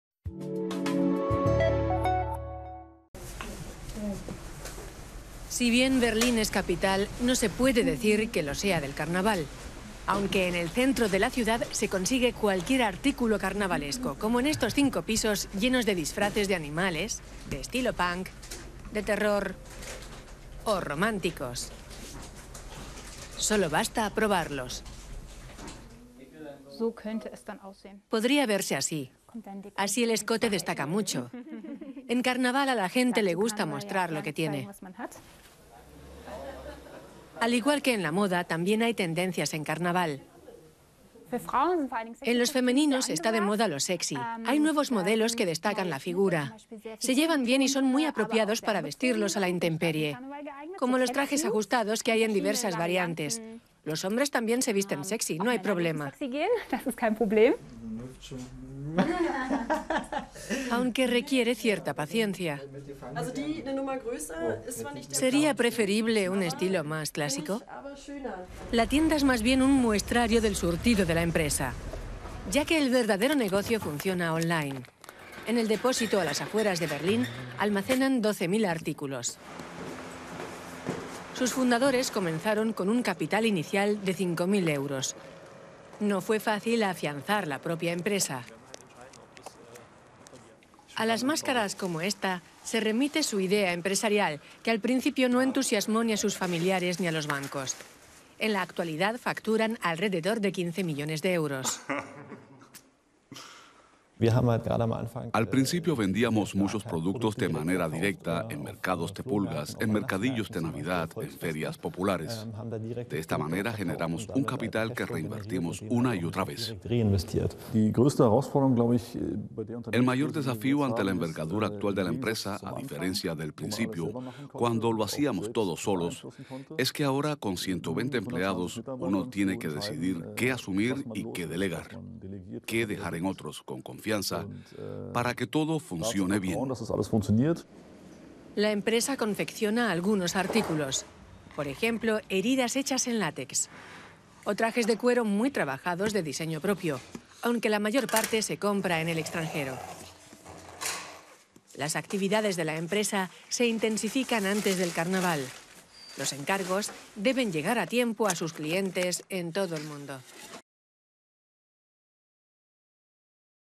Alemania y el negocio del carnaval
En épocas previas al carnaval se incrementa la actividad comercial de Maskworld. Esta empresa con sede en Berlín se ha especializado en producir y distribuir artículos de carnaval como disfraces, máscaras y demás accesorios. Su negocio va viento en popa.